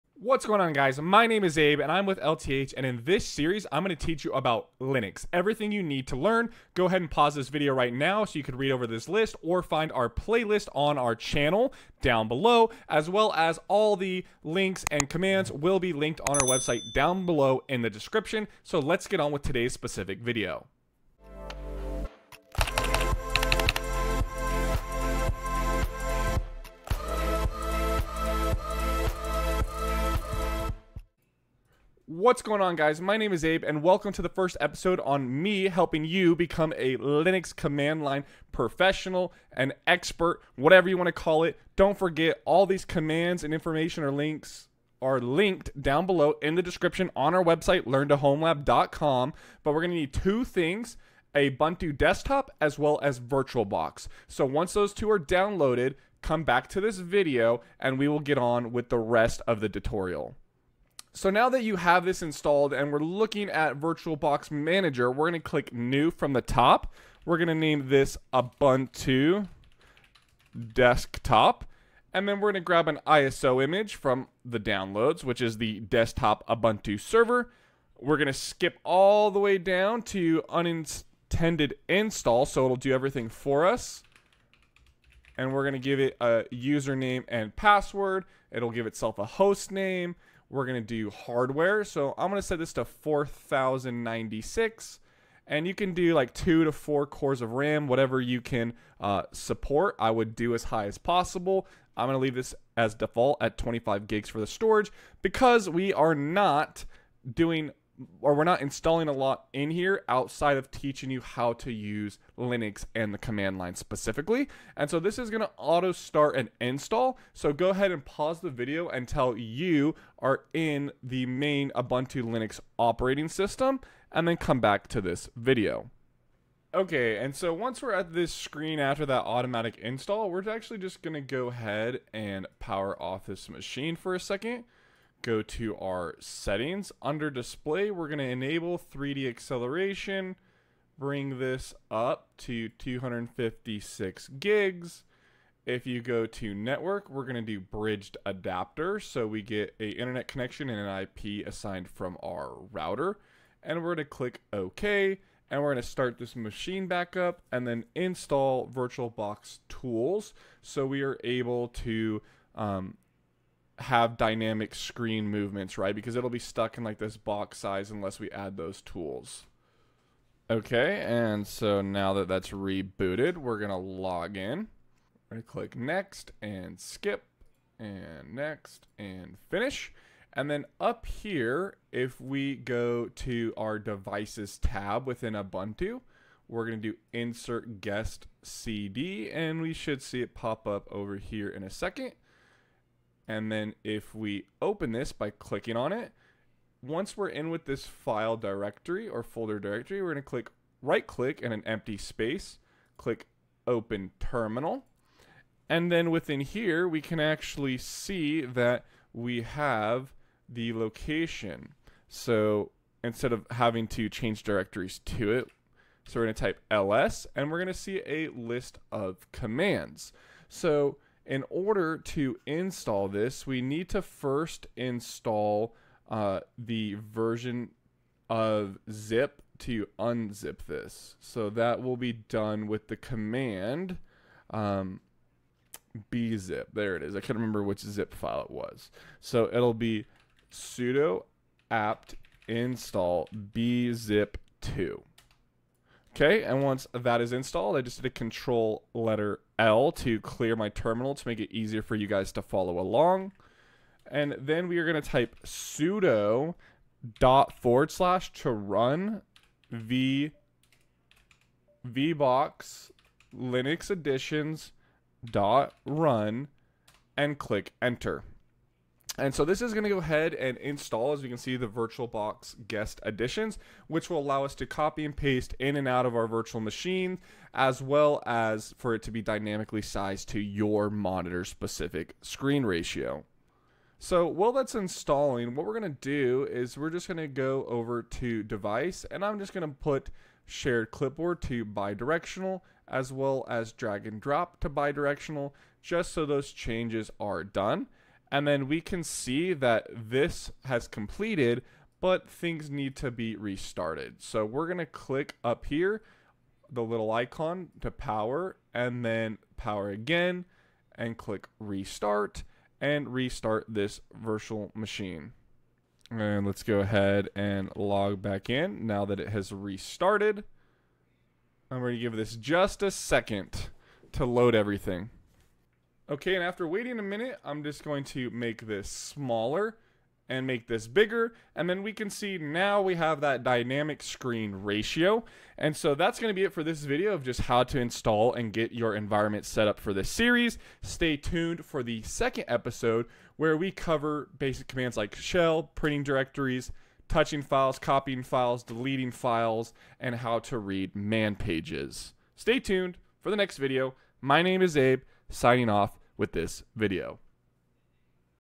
Can You Learn Linux In One Day?
Hey guys, Abe here; please see the links below to find out more info on this video or related content on our channel. This video is a Linux tutorial for beginners, where you'll begin your Linux journey and learn Linux commands! We will be covering the Linux command line and how to use the Linux terminal, all using Ubuntu.

Timestamps:
00:00 Intro
00:33 Software for our Linux Environment
01:00 Installing Ubuntu Desktop in VirtualBox
02:45 How to Install VirtualBox Guest Additional Tools on Ubuntu Desktop
05:35 Outro on how to Set up your Linux Environment